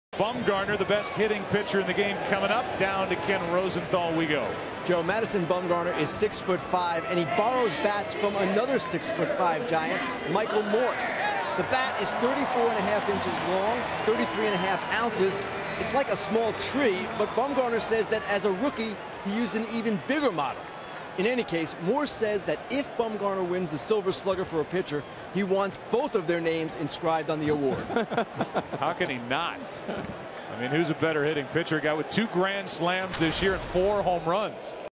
NLCS Gm5: Bumgarner uses Morse's lumber during AB
10/16/14: Madison Bumgarner steps into the box with Michael Morse's bat in the 3rd inning